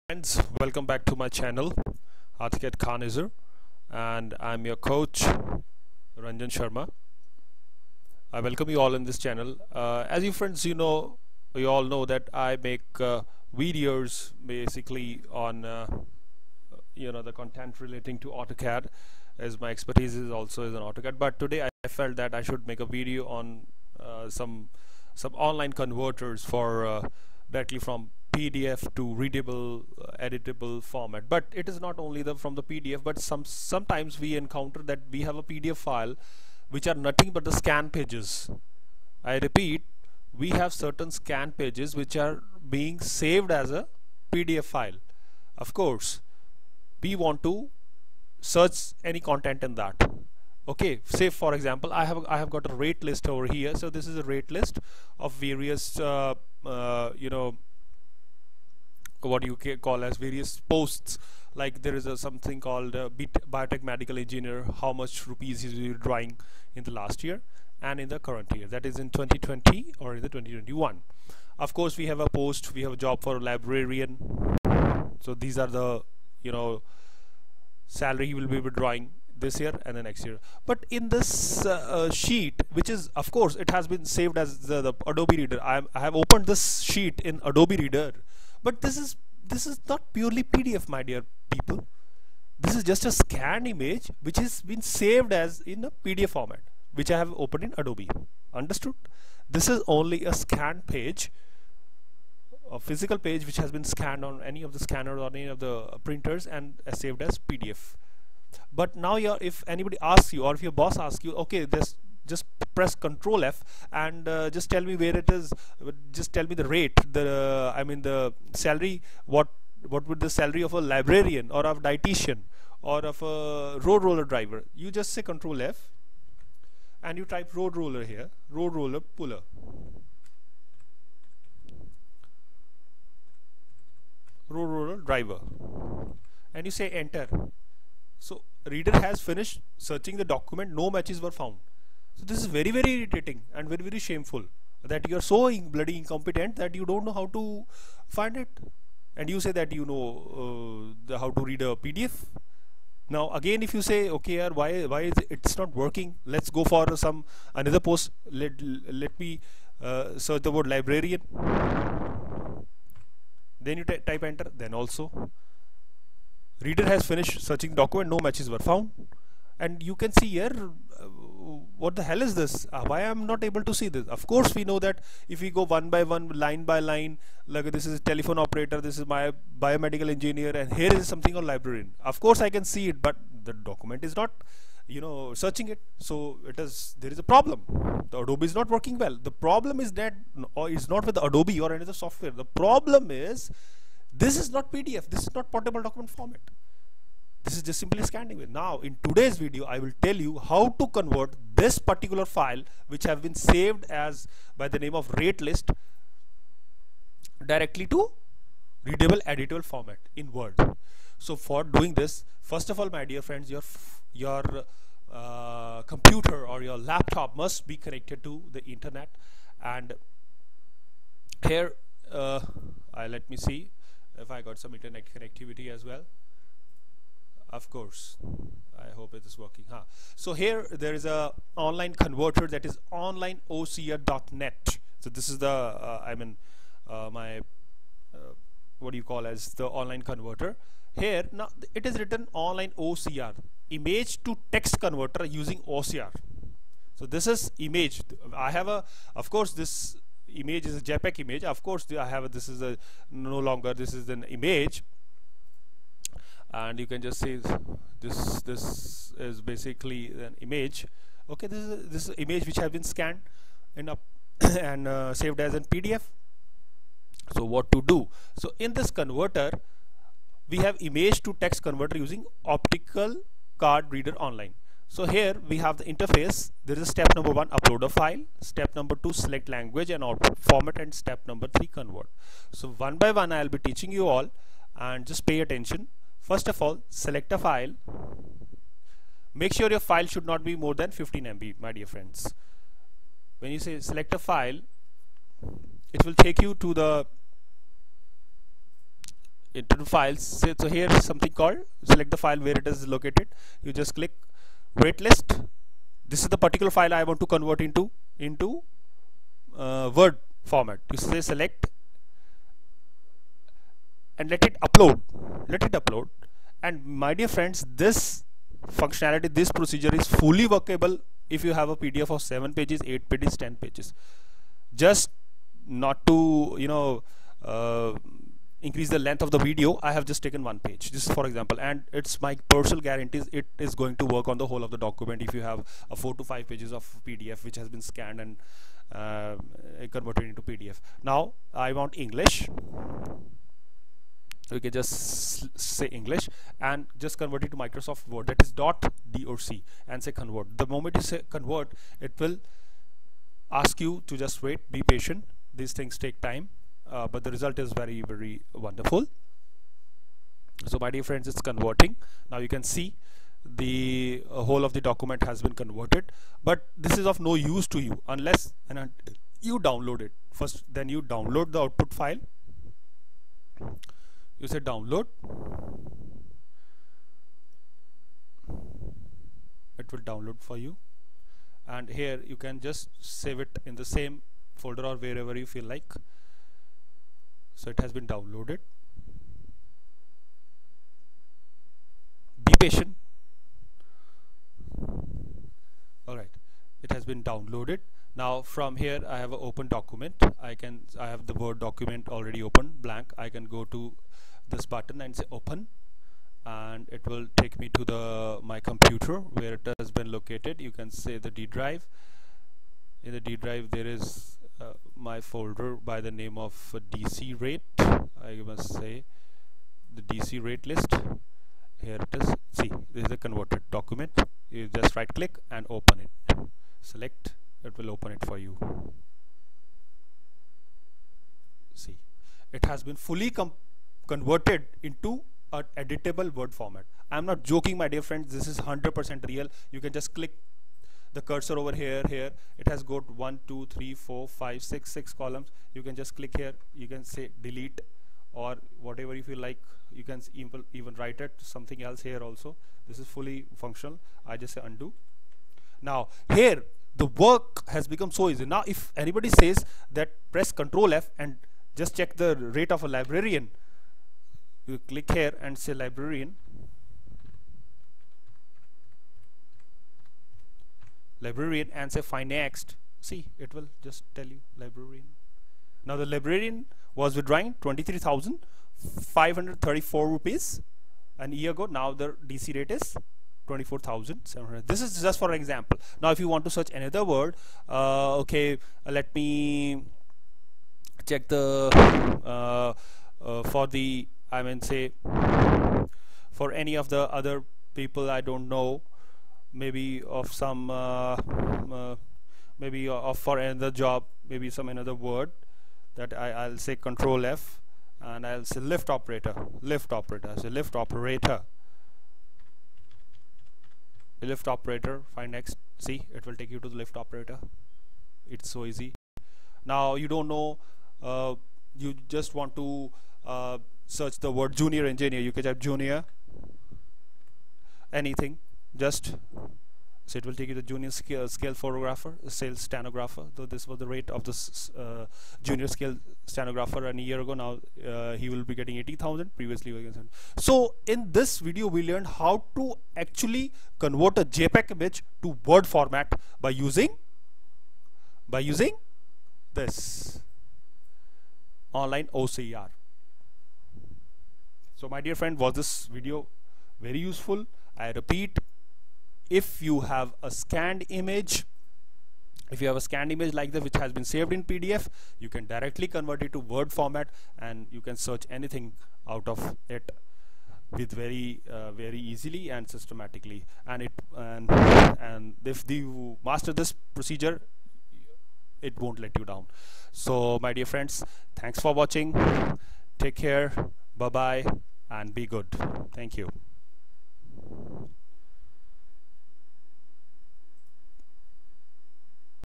How to convert Text JPEG images to Word doc Format
Now anyone can easily convert the jpeg images to editable word format i.e from .jpeg to .doc format. Also, the scanned images which are saved to pdf can be easily converted to editable word .doc using this online conversion tool.